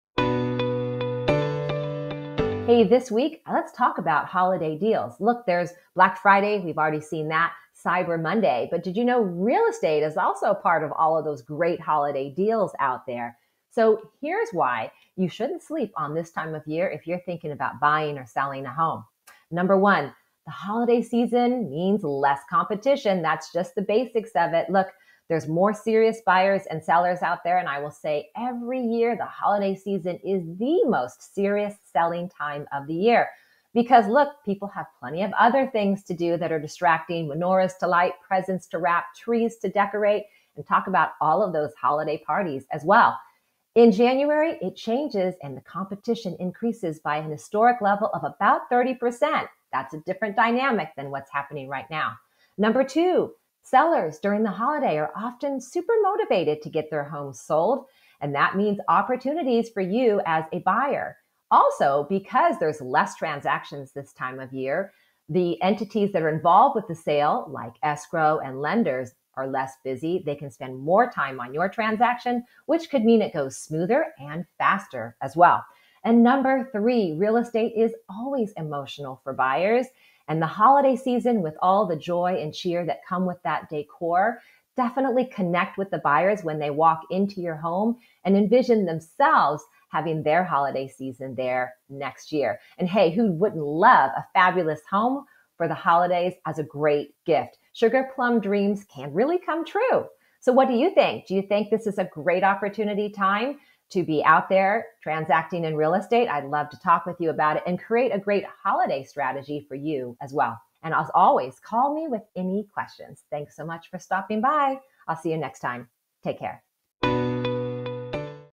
How to Get HOLIDAY DEALS in Real Estate
Black Friday, Cyber Monday, and Real Estate? 🧐

Let's talk holiday deals!

Today we are talking about why you shouldn't sleep on this time of year if you're looking to buy or sell a home ... It's the most wonderful time of the year AND it's the most SERIOUS SELLING TIME OF THE YEAR. 🤓 This week's episode explores the opportunities in real estate you don't want to miss! 👌🏻

Plus who wouldn't want a fabulous new home for the holidays?? 🎁🏡

Check it out, then call the best realtor you know! 🙋🏼‍♀️

Michelle Wilson
SearchSouthCounty.com
Realtor Cal BRE#01870068
First Team Estates | Christie's International